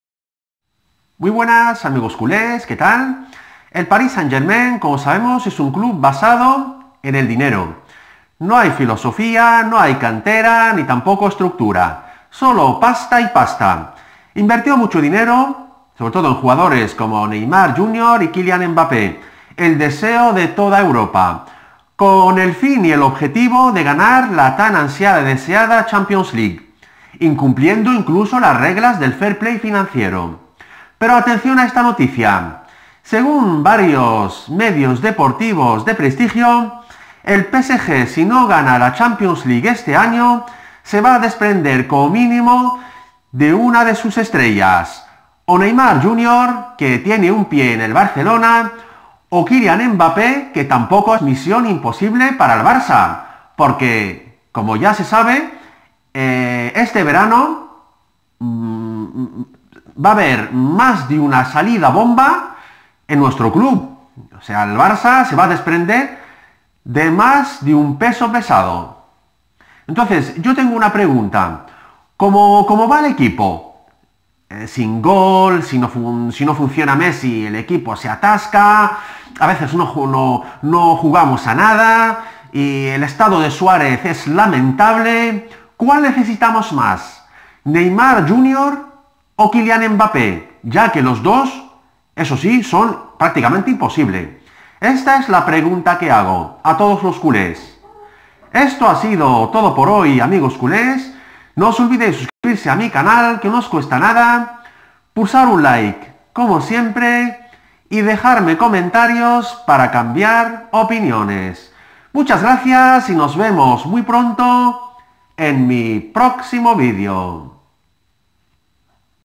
NEYMAR O MBAPPÉ AL BARCELONA
El Barcelona irá a por neymar o Mbappé si el psg no gana la champions league